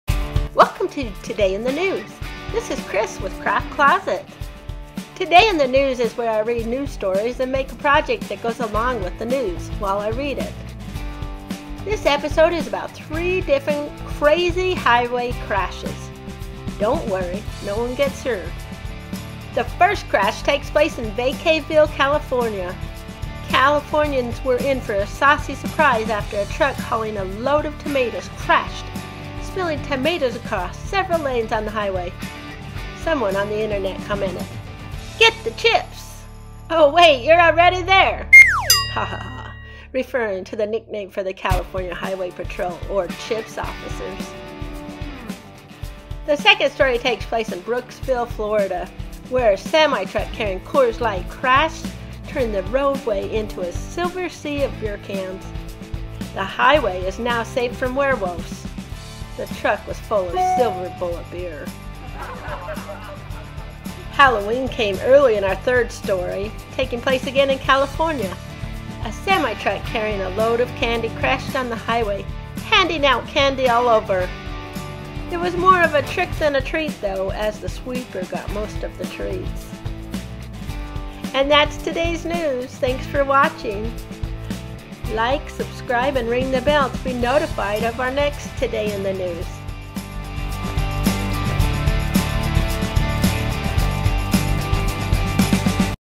Today in the News: Crazy Crashes
Join us for Today in the News!  This is where Kris makes a project and tells us new stories! Today's episode is about 3 crazy truck crashes!